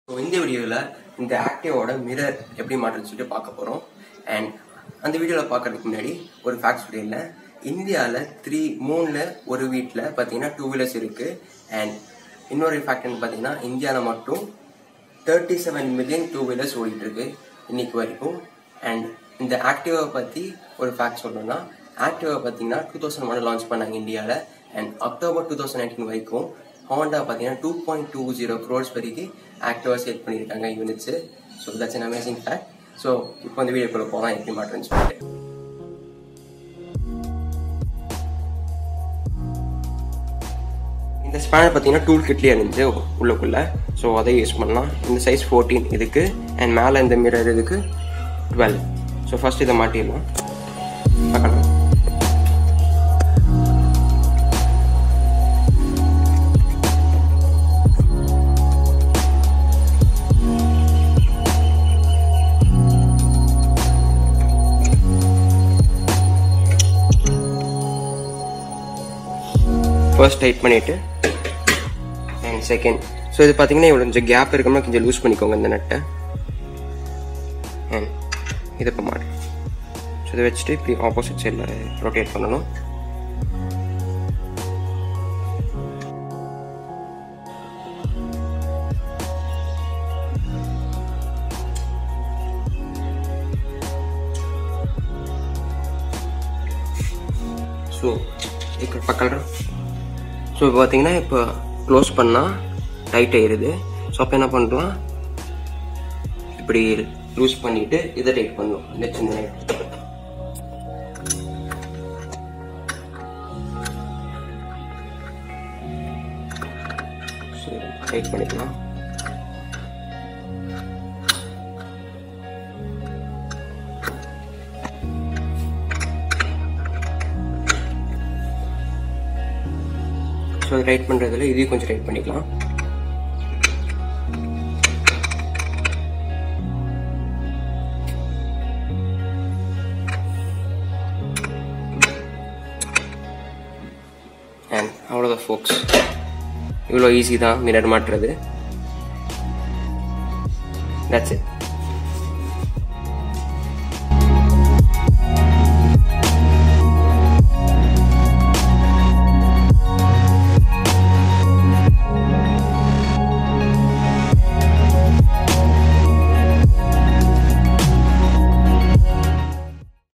How to Fix Side Mirrors In Any Scooter Ft Honda Activa 5G
This video shows you how to fix side mirrors in any scooter. In this case i have used honda activa 5g & shown the steps to fix it .
Thanks a ton for spending your valuable time and watching my video.

How to Fix Side Mirrors In Any Scooter